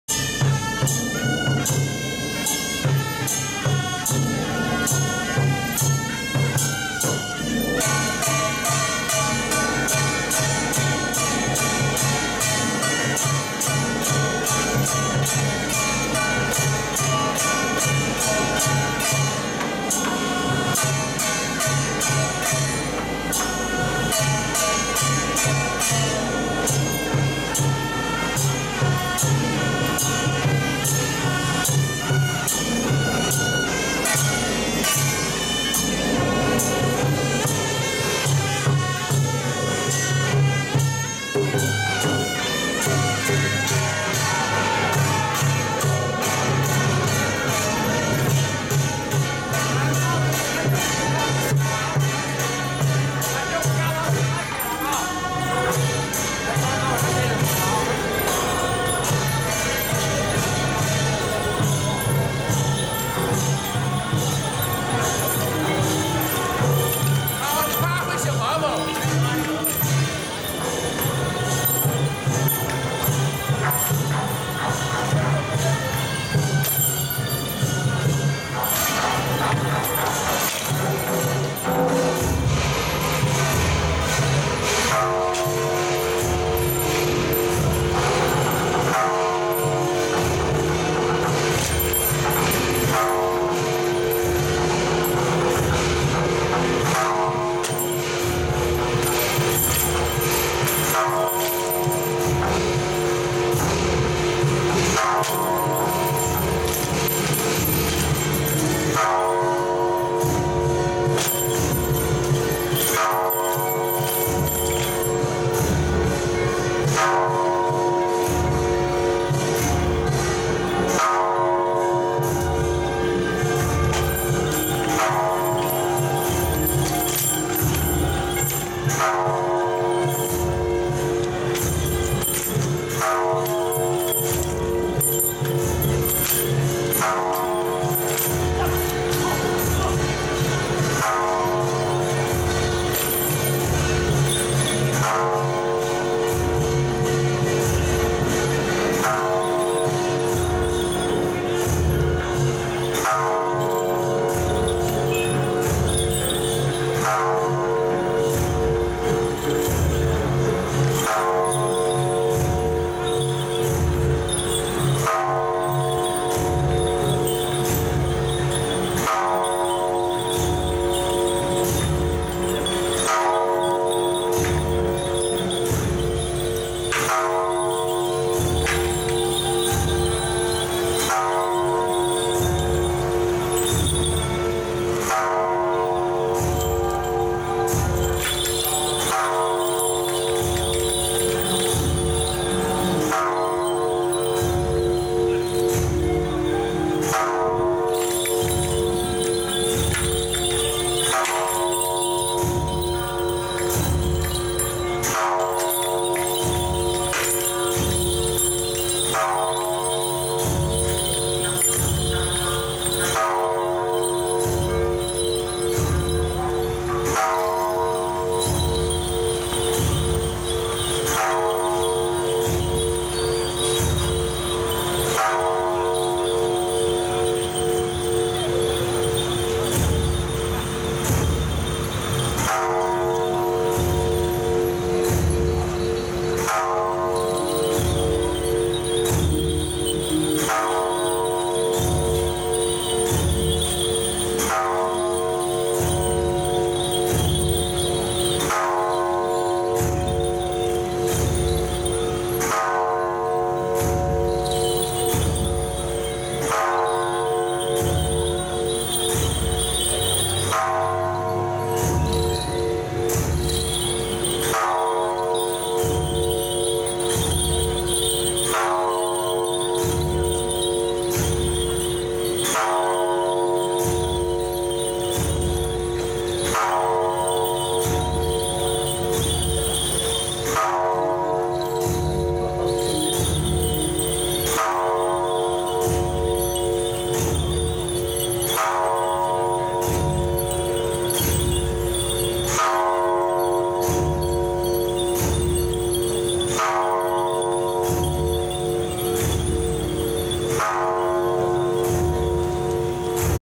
108.1.13北廠金勝宮恭送天師回鑾遶境6【安平興和壇駕前什家將】
2019/1/13府城崇福宮境北廠金勝宮慶成祈安三朝建醮大典恭送天師回鑾遶境
↓ ↓ ↓更多相關影片 ↓ ↓ ↓